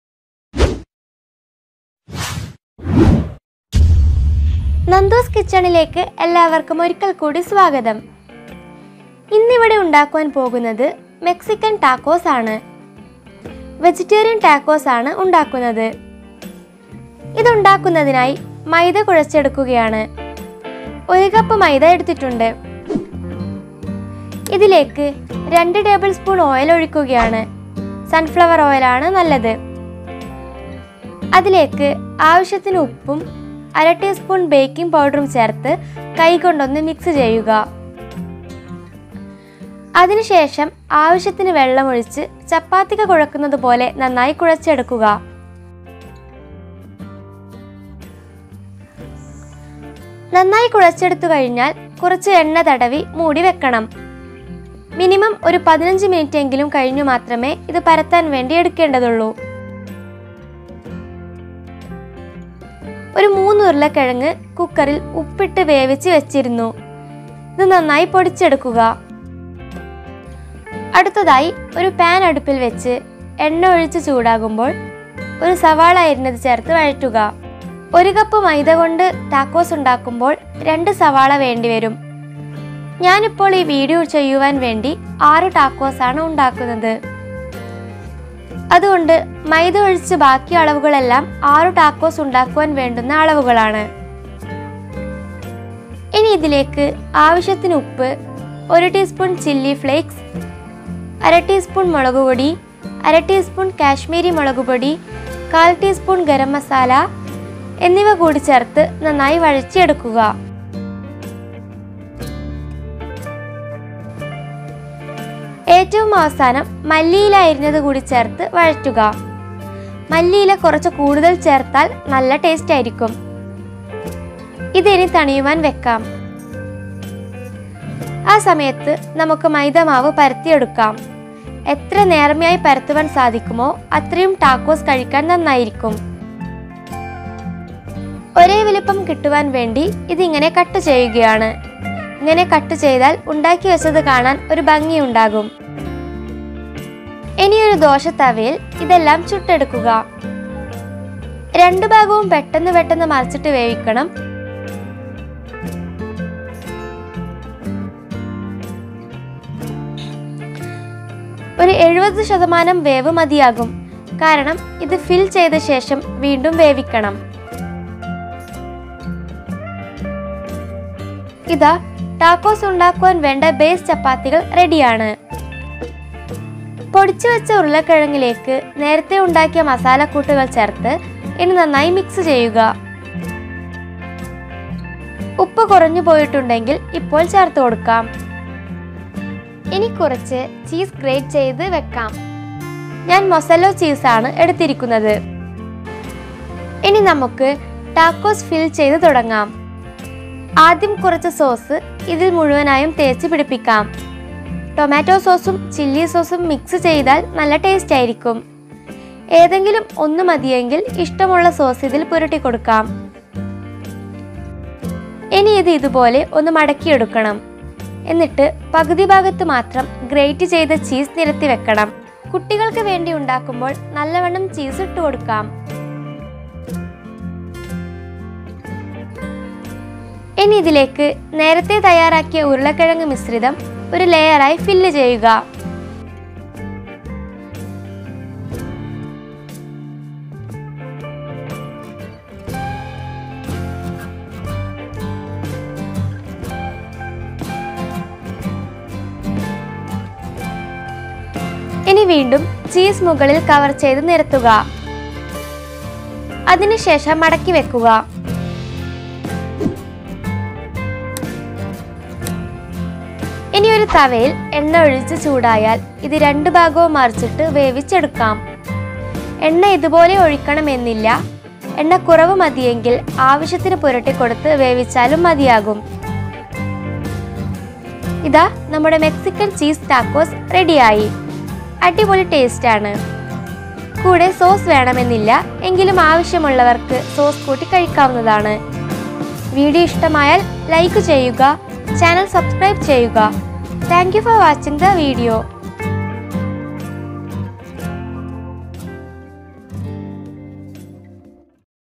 Homemade Mexican Cheese Tacos Potato Tacos മെക്സിക്കൻ ടാകോസ്
How to make Tacos

Nandus Kitchen Malayalam  YouTube Channel
Makes several cooking videos
This is a video that shows you how to make Tacos in a very simple way

Any suggestions and enquiries please contact